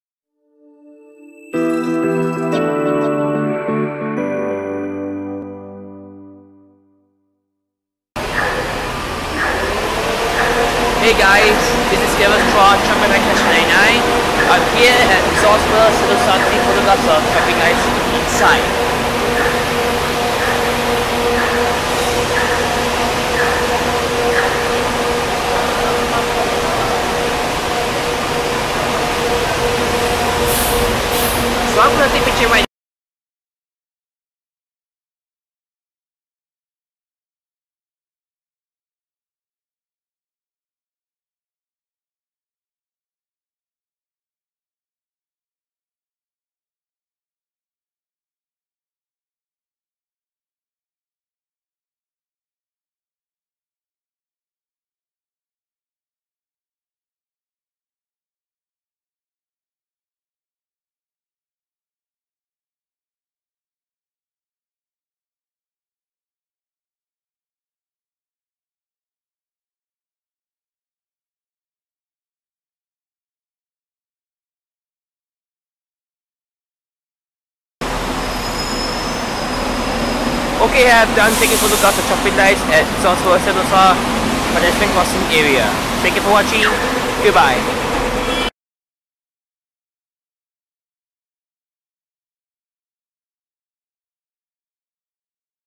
Resorts World Sentosa Traffic lights
This video is to take photographs of traffic lights at Resorts World Sentosa. Taken on 5 December 2017.

Camera photographed/videographed by
NIKON D5300 & COOLPIX S6900